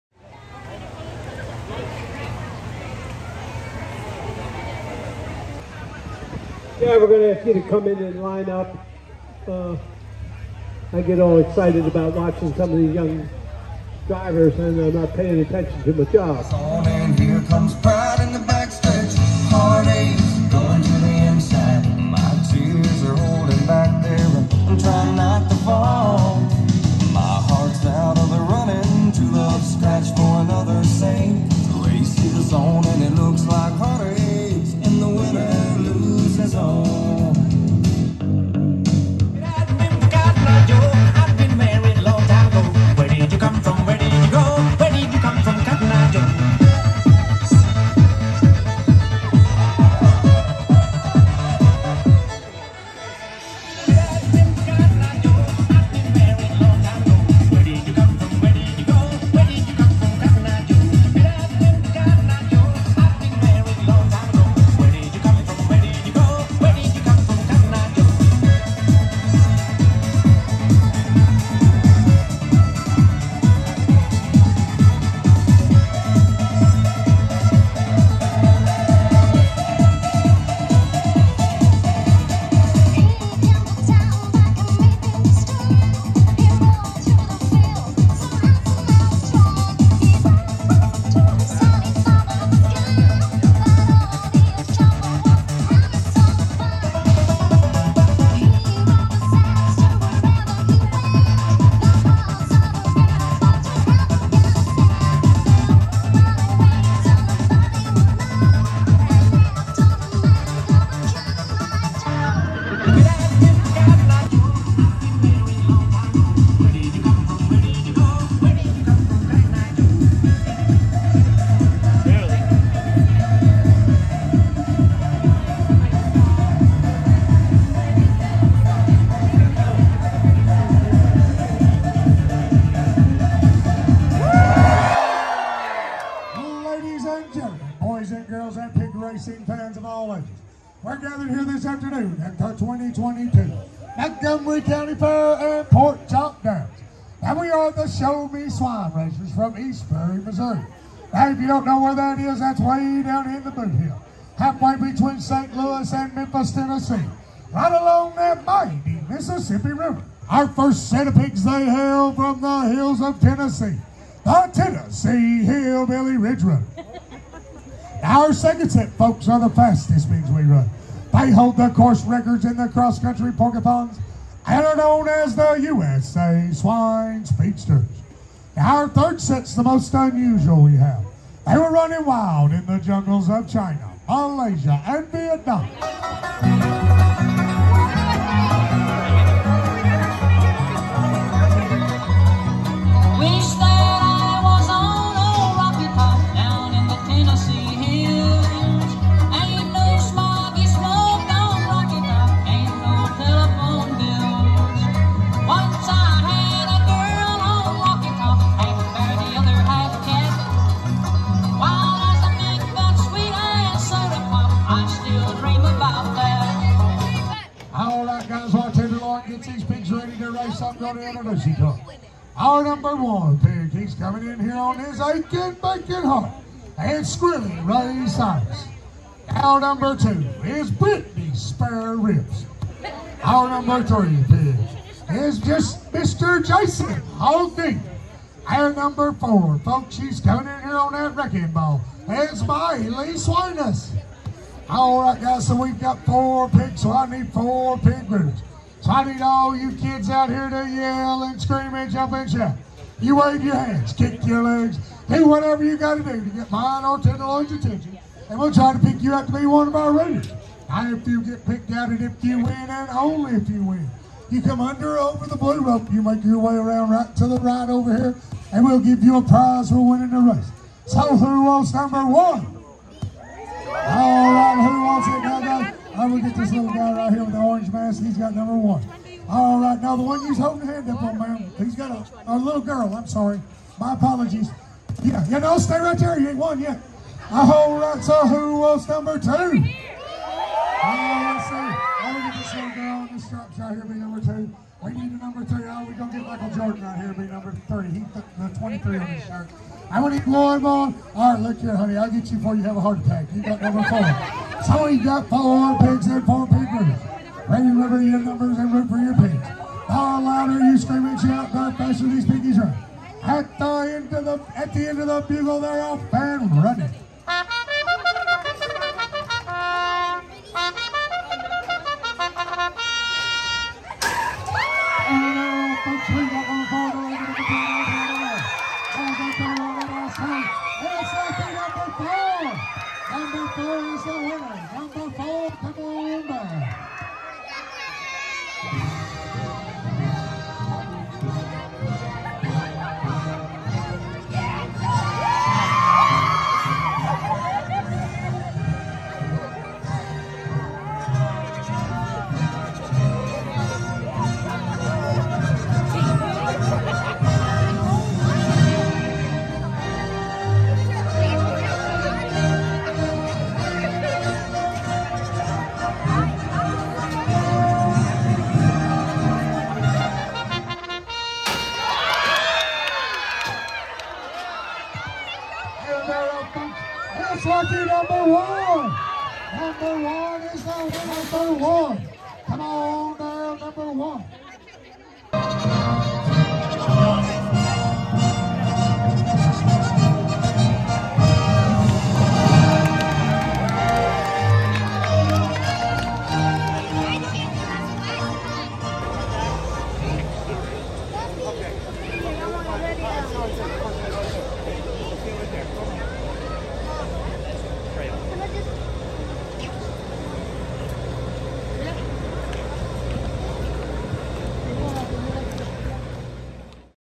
Montgomery County Fair 2022 Part III
I visited Montgomery County Fair 2022, I also cheered on the race of cute little pigs.

Keep supporting 👍🏼❤️🙏